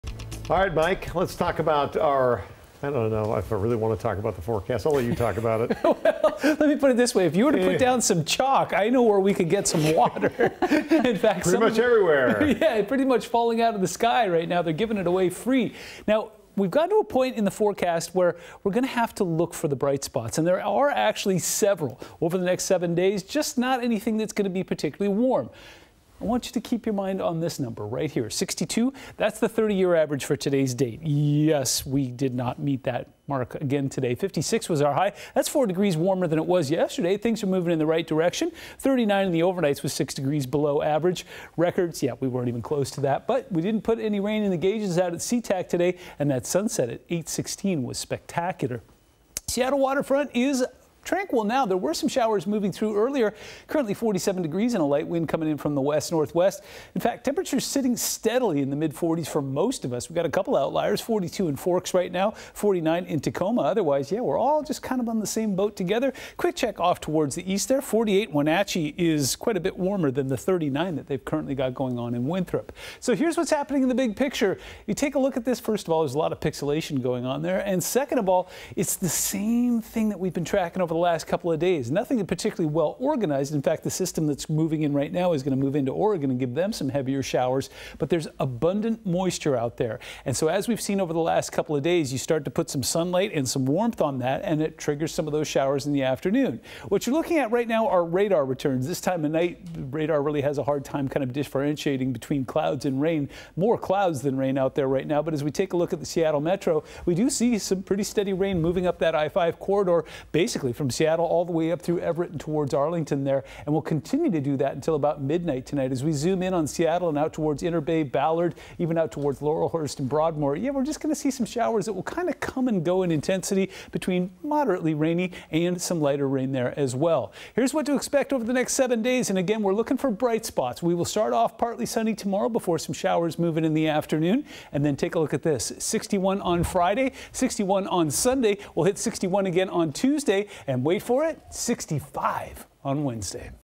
KING 5 Weather
KING 5 Meteorologist Mike Everett gives an update on the late evening forecast for 04/27/22.